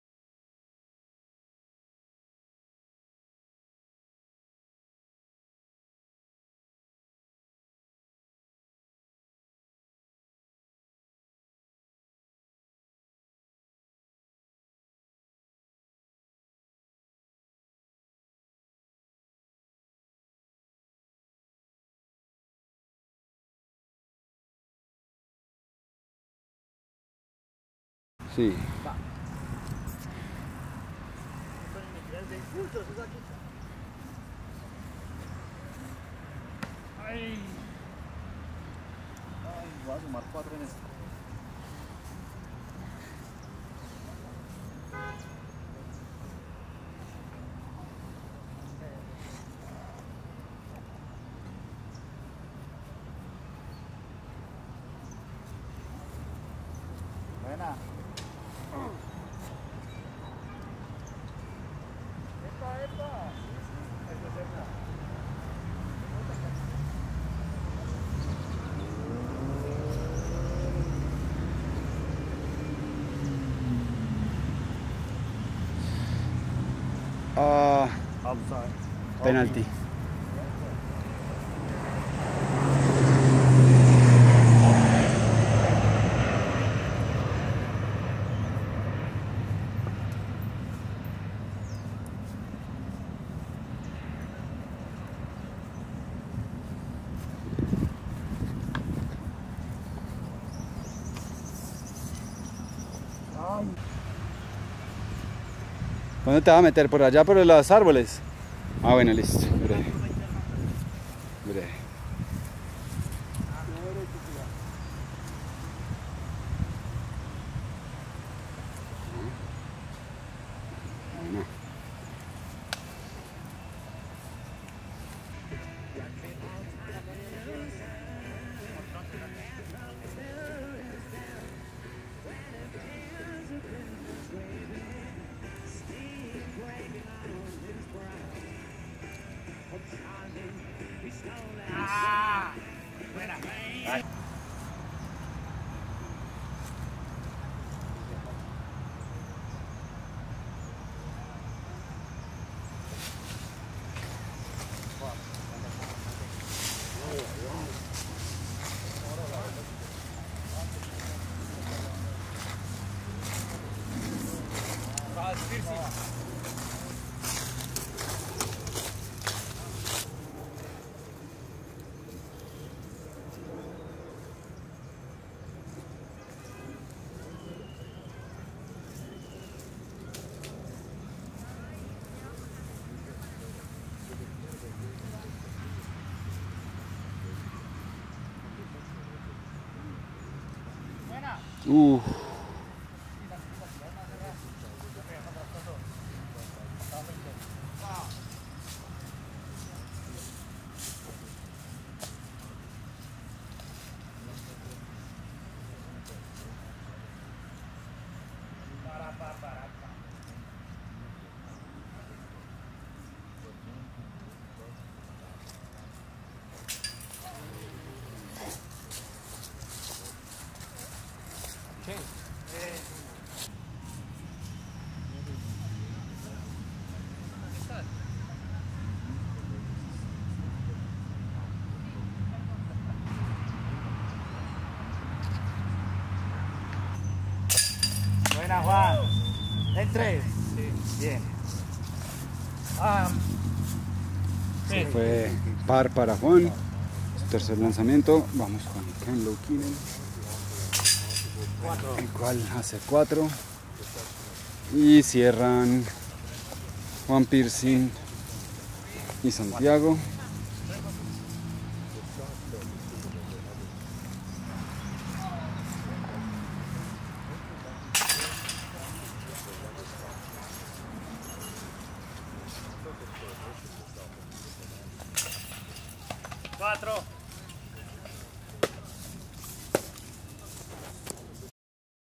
9B - Aero Park Disc Golf JP2 - CESTA 10
Aero Park Disc Golf JP2 - Evento de disc golf en el aeroparque Juan Pablo II.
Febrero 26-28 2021